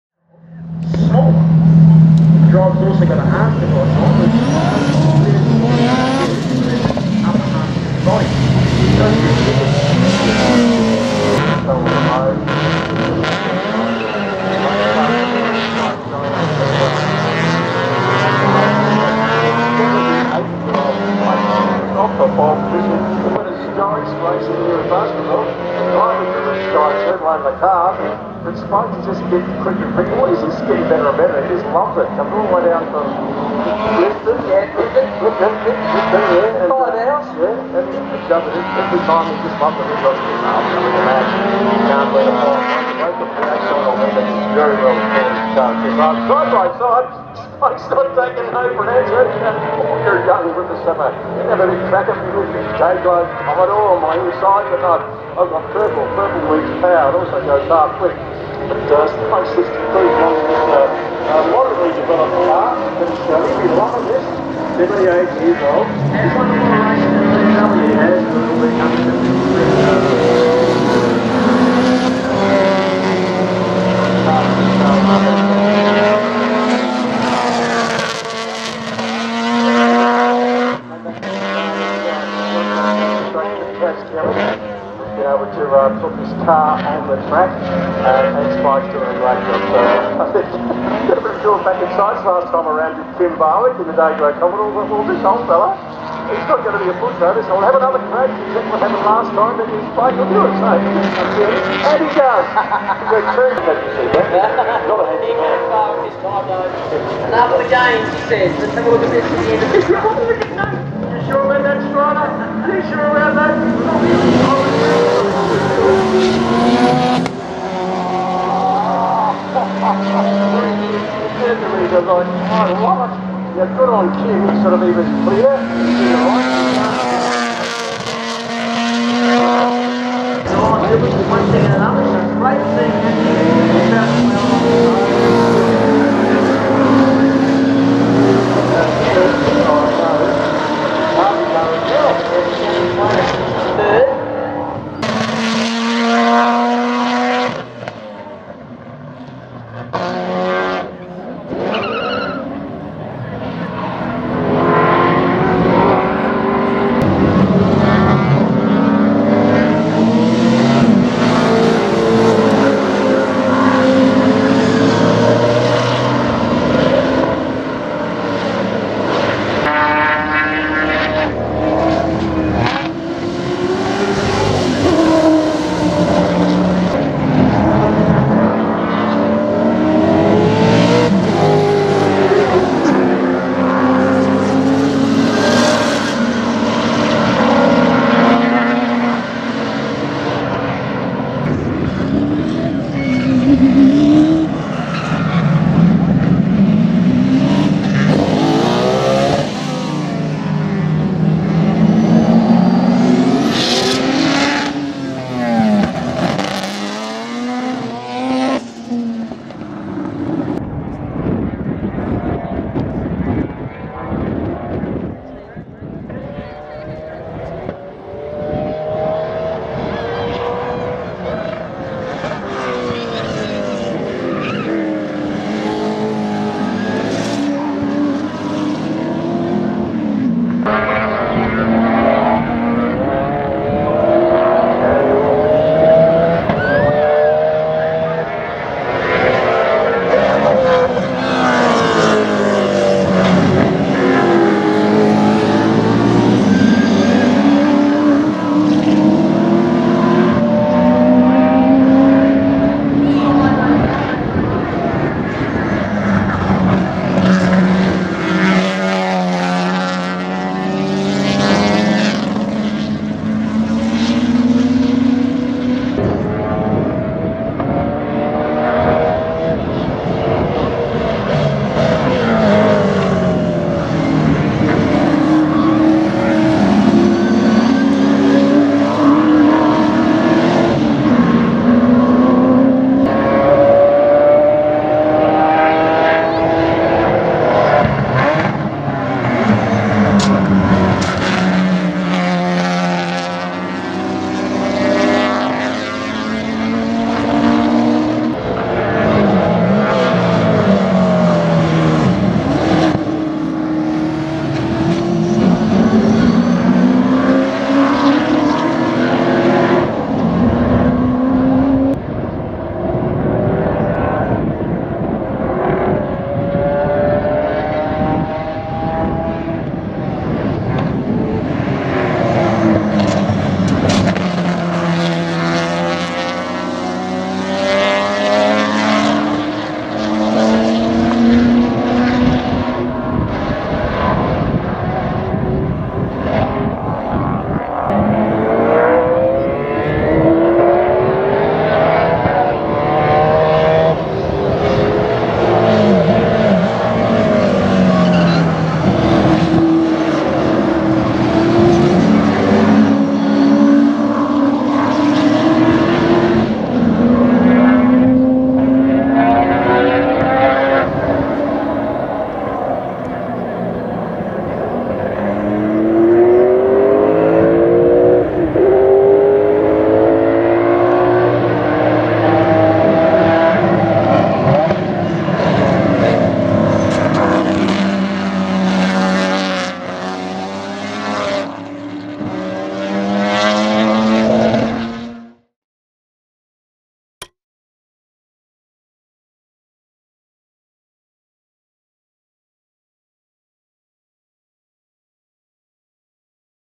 Sports Sedans Races 1 & 2 Baskerville Historics 2022 | NASCAR powered VK Commodore V8 big banger
I had some dramas with my camera during race 1 so a lot of the clips may seem jerky or not stabilised properly.